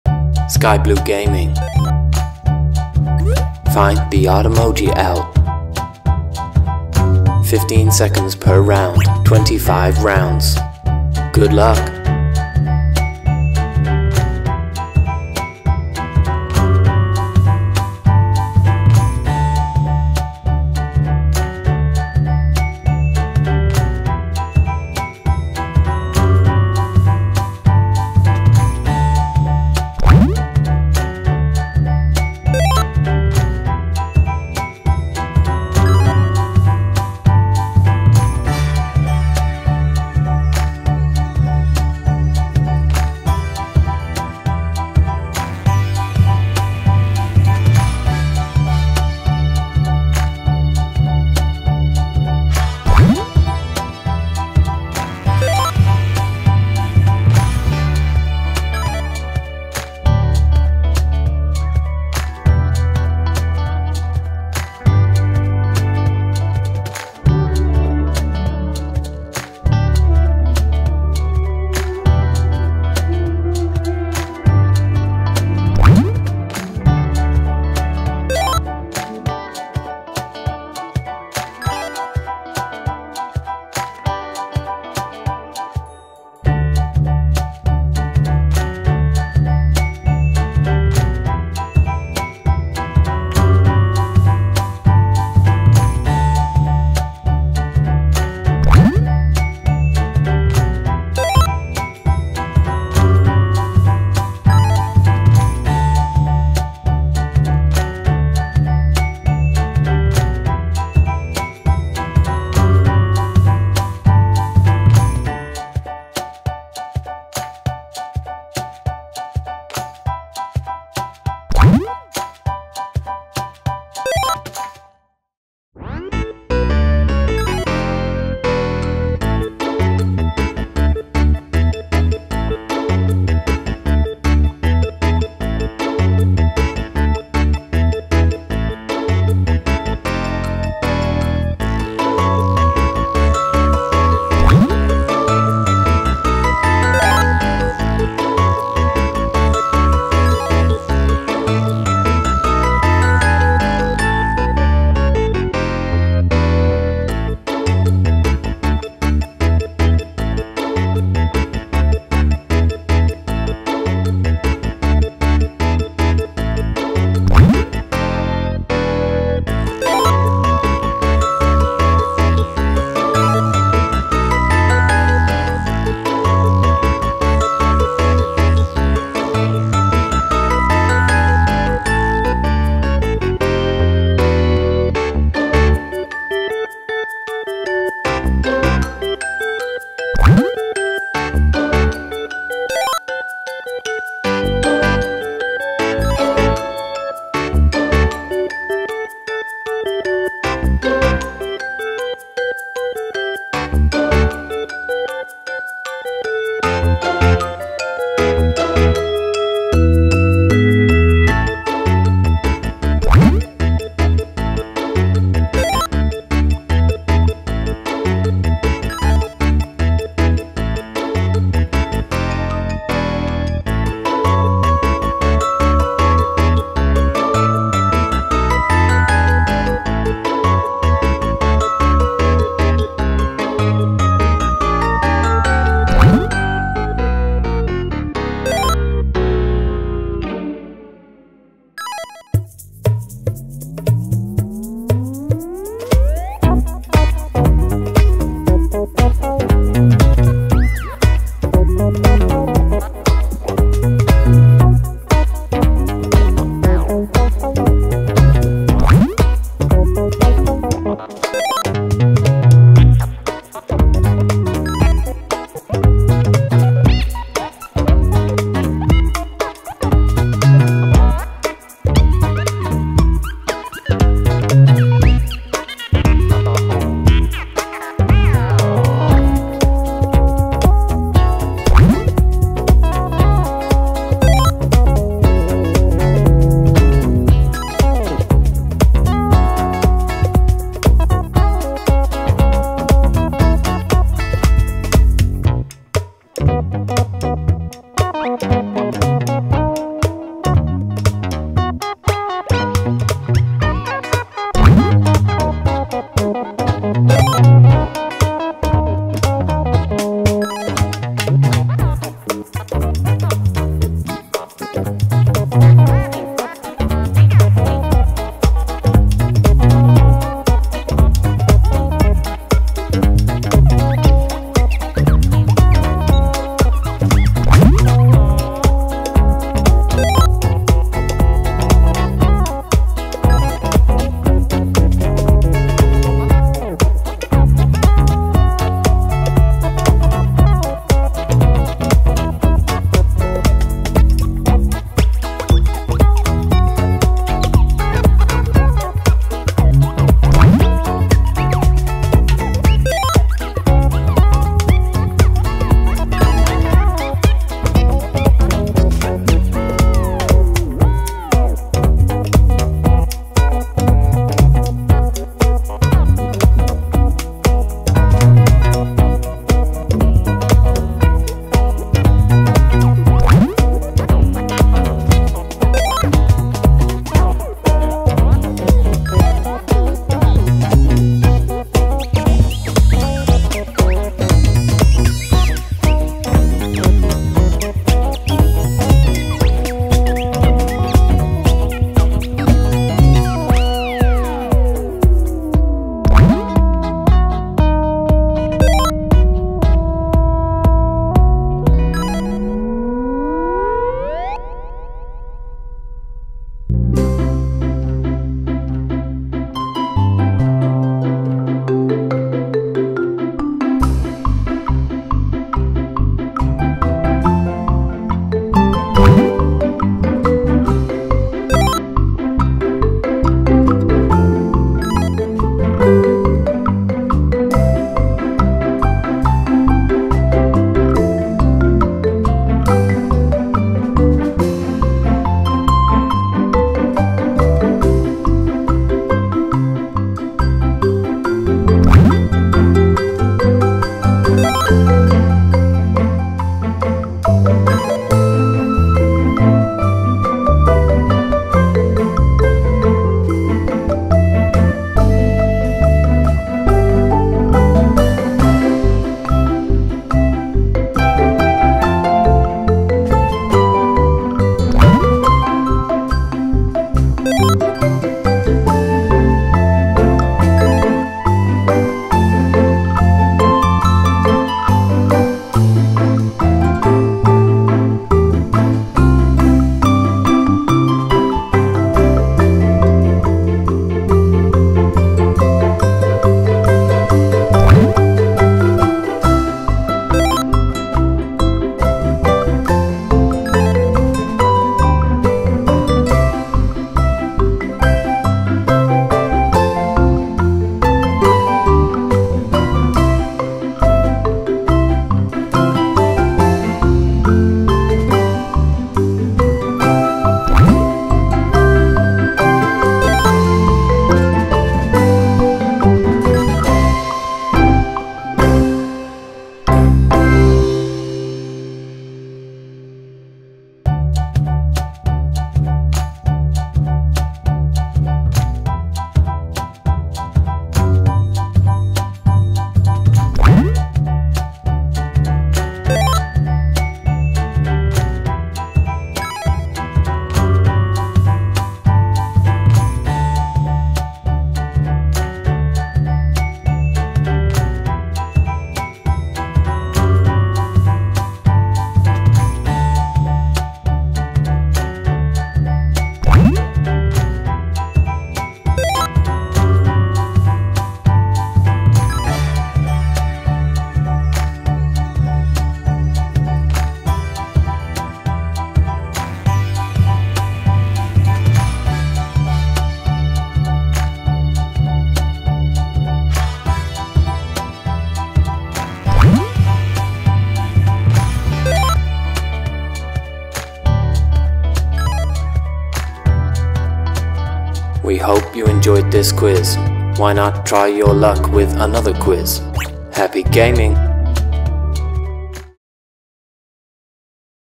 FIND THE ODD EMOJI OUT by Spotting The Difference! | Odd One Out Puzzle | Find The Odd Emoji Quizzes
Welcome to Sky Blue Gaming. Can You Find The Odd Emoji Out in 15 seconds? See how good are your eyes with this family fun emoji quiz. You have 15 seconds to find each of the odd emojis. There are 25 rounds in this emoji game to complete.
Good Luck!

If you fancy playing some more games here are some we can highly recommend.
☀️ Hands Down
☀️ Uno Emoji
☀️ Bop it!
☀️ Pass the Potato
☀️ Emoji Stamps